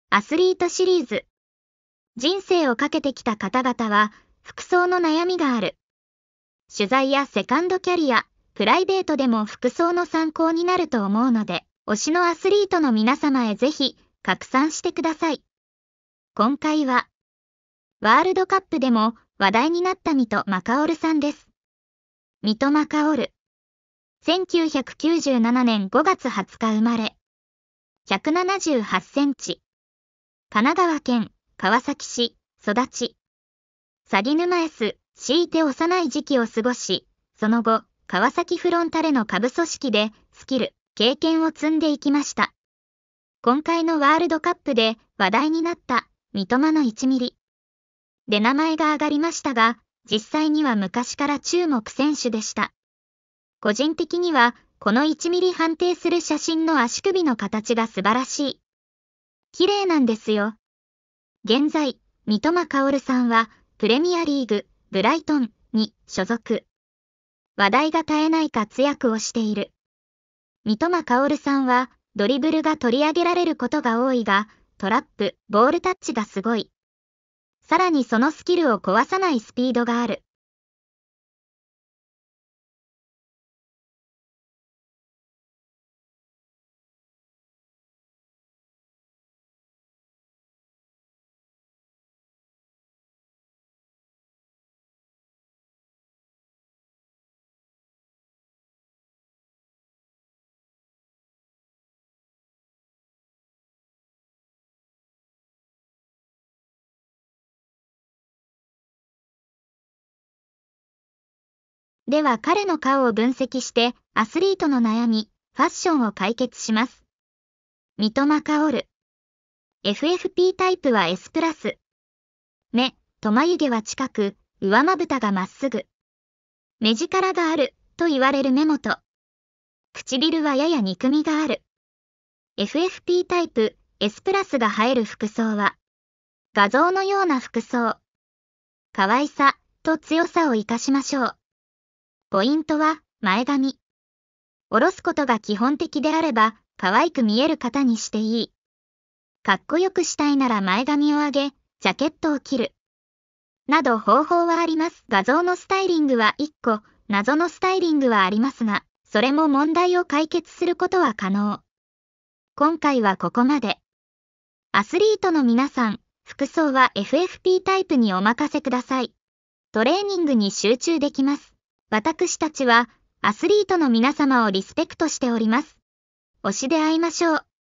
スーパーゴール量産中！三笘薫の顔と服♡
For Kaoru Mitoma's success. dress someone else♡
Kaoru Mitoma is currently playing an active role in the Premier League!
Keep an eye on future trends!
I want him to focus on training, so I support his outfit.

Getting people to wear trendy and new items is just what brands do.
Then the earth cannot be saved.

Leave it to us [FFP type] who use [face and clothes].

三笘薫、現在プレミアリーグで大活躍中！
今後の動向に注目だ！
彼にはトレーニングに集中してほしいので、彼の服装を支えていく。

トレンドや新作を着てもらうのは、ただのブランドがやる事。
それでは、地球は救えない。

【顔と服装】を使う、我々【FFPタイプ】にお任せください。